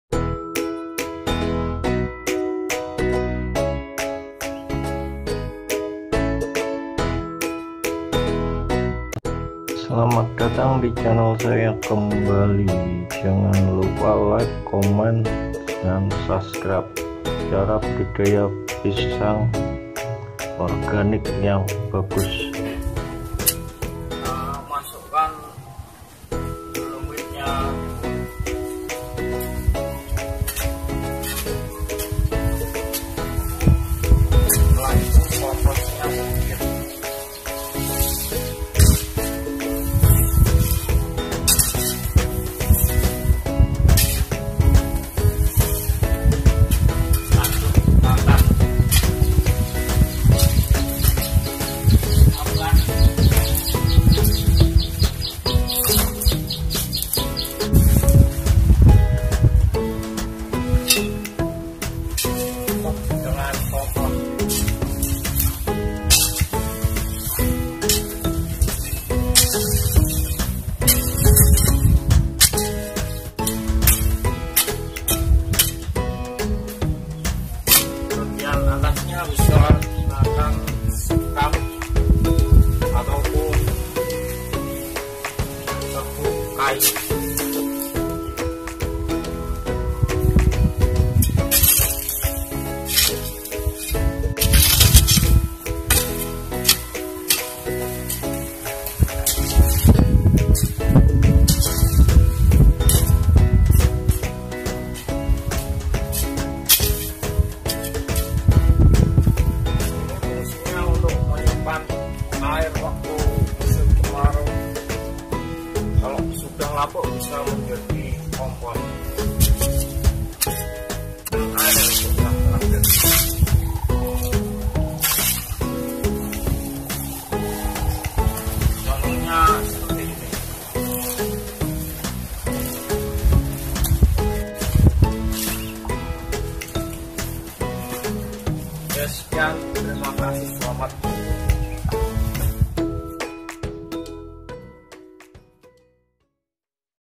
Rahasia Budidaya Pisang dilahan Kering ! Cara Tanam Pisang Organik Cepat Berbuah
Budidaya Pisang Organik Bagus Cepat Berbuah Besar. Cara Lengkap Tanam Pisang dengan metode baru. Rahasia Pisang Organik Cepat Cepat Berbuah Sepanjang Musim, media tanam pisang yang bagus akan menjaga nutrisi dan menyimpan air saat musim kemarau..